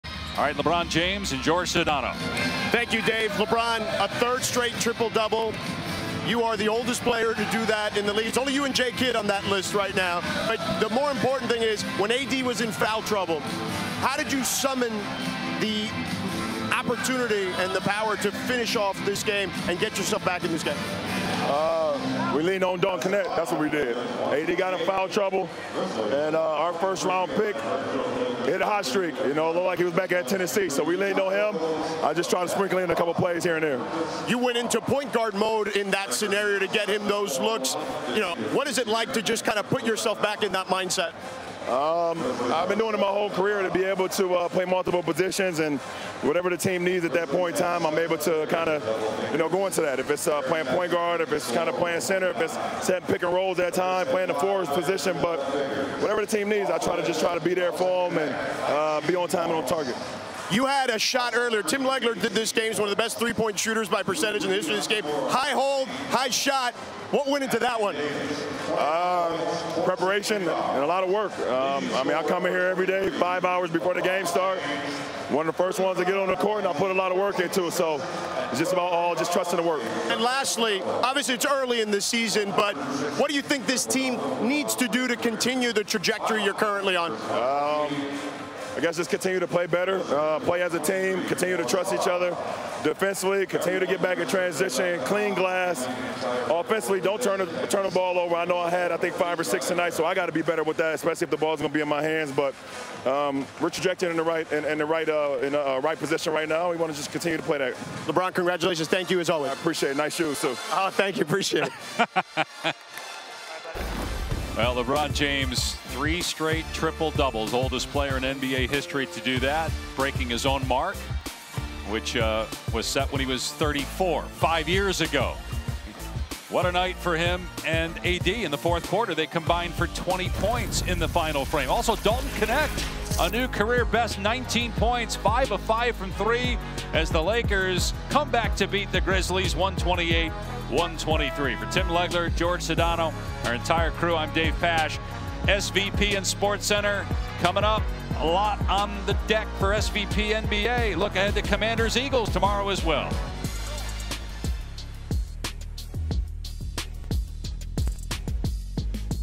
LeBron James reacts to 3rd Straight TRIPLE-DOUBLE & Win vs Grizzlies, Postgame Interview
Memphis Grizzlies vs Los Angeles Lakers - Full Game Highlights | November 13, 2024 | 2024-25 NBA Regular Season